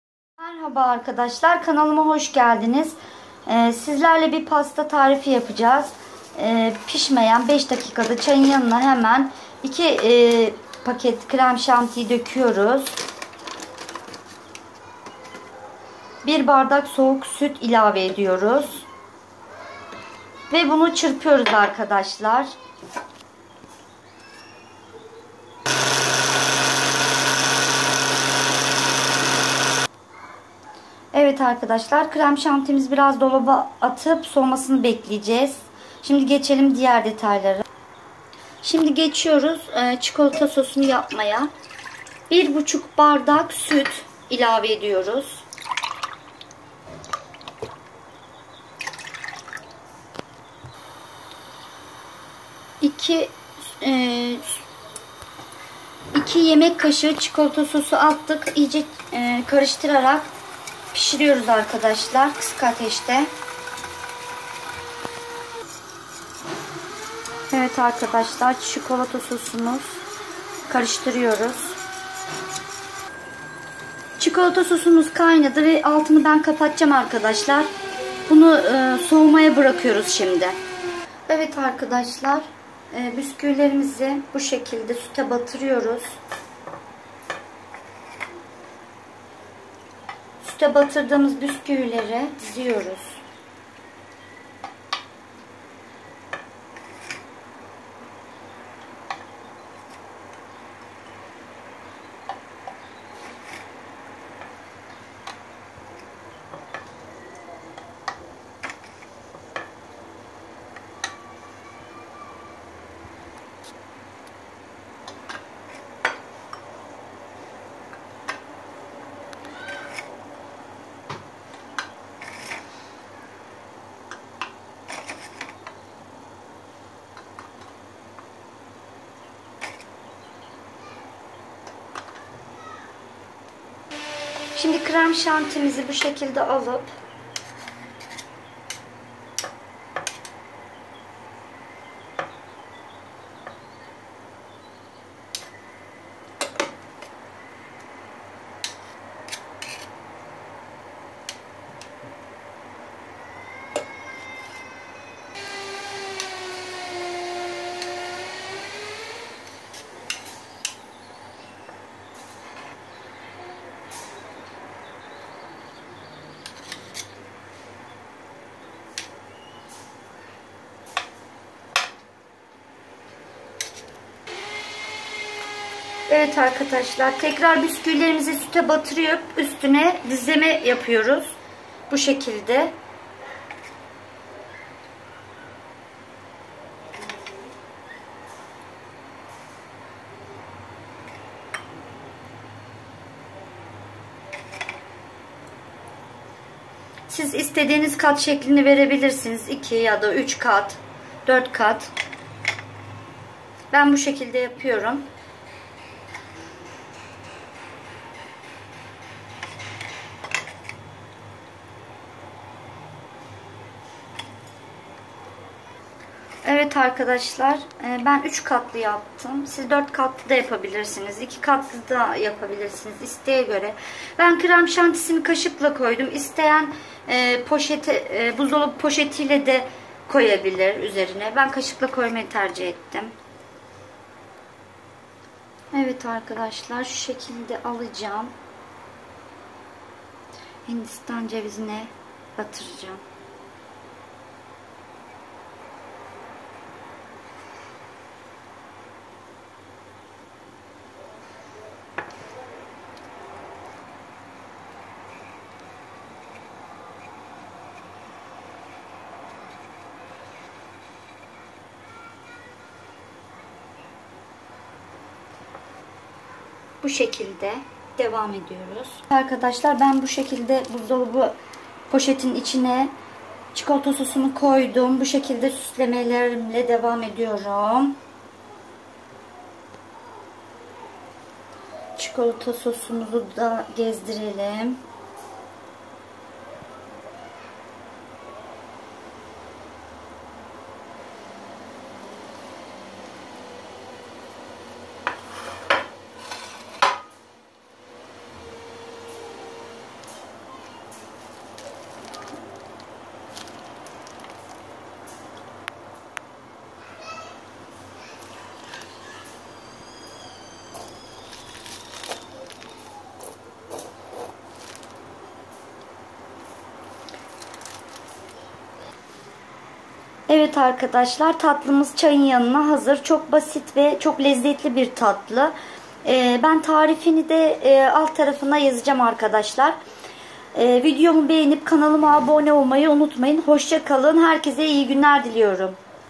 5 DAKİKADA PİŞİRMEDEN YAPILAN MÜTHİŞ TATLI TARİFİ
Malzemeler
*2 paket krem şanti
*1 buçuk şu bardağı soğuk süt
*3 paket kakaolu bisküvi
*süt (bisküvileri ıslatmak için)
Süsleme için
*Hindistan cevizi
*çikolata sosu